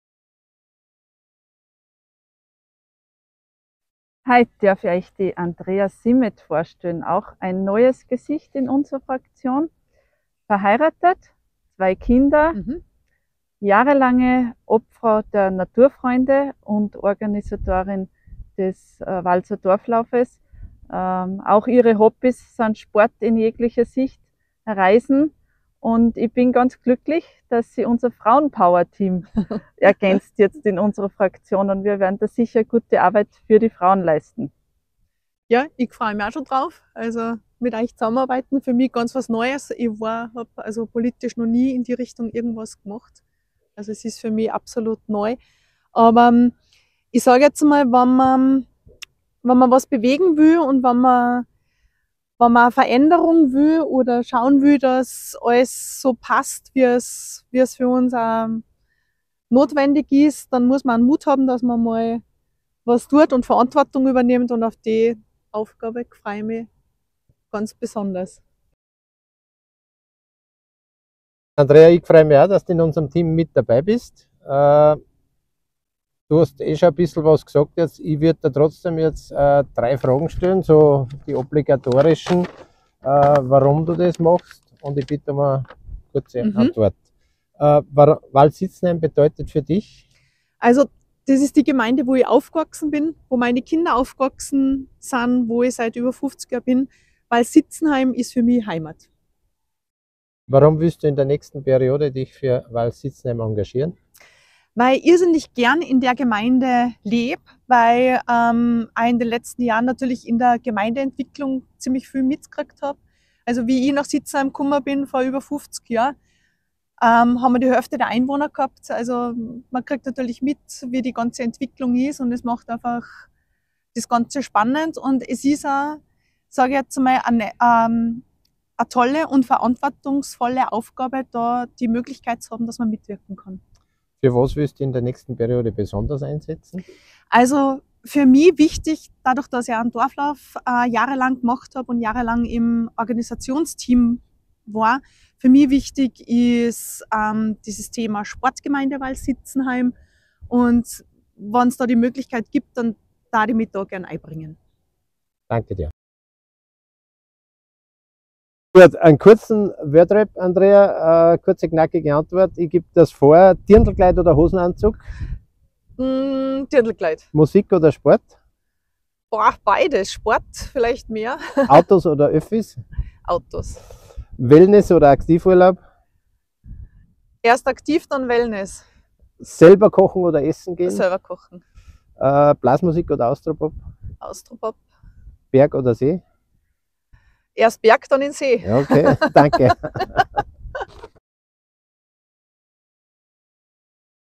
Kandidatin Nr 14: Andrea Simeth
Andrea Simeth
Platz 14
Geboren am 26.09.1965
Verheiratet
Eine Tochter, einen Sohn
Assistentin Wirtschaftskammer | Sparte Tourismus
Ehrenamtliche Funktionen und Mitgliedschaften bei Vereinen/Institutionen:

ehemalige Obfrau Naturfreunde Wals
Organisatorin des Walser Dorflaufs
Wofür will ich in der Gemeinde Verantwortung übernehmen?

Nach über 30 Jahren im Tourismus bei verschiedenen Reiseveranstaltern habe ich mich während der Zeit der Pandemie für einen neuen Tätigkeitsbereich entschieden und bin in die Wirtschaftskammer Salzburg gewechselt. Zu meinem jetzigen Aufgabenbereich zählen u.a. die Organisation der Veranstaltungen der Sparte Tourismus und Freizeitwirtschaft, wie das Symposium Zukunft:Tourismus 2023 im Karl Böhm Saal des Festspielhauses, das Fest der touristischen Lehre in der Residenz zu Salzburg und die Berufsinformationsmesse im November.

Ich bin Quereinsteigerin und hatte noch nie eine politische Funktion. Meine Kompetenz liegt darin, Mut zur Verantwortung zu haben und an Herausforderungen selbstbewusst und mit Hausverstand heranzugehen. Viele Jahre habe ich mit meinem Team den Walser Dorflauf organisiert und wurde immer von unserer Gemeinde in allen Bereichen bestens unterstützt.

Der Tourismus und der Sport sind seit vielen Jahren ein wichtiger Teil meines Lebens, daher möchte ich gerne Verantwortung für die Sportgemeinde Wals-Siezenheim sowie für den Tourismus in unserer Gemeinde übernehmen und dafür meine größtmögliche Unterstützung leisten.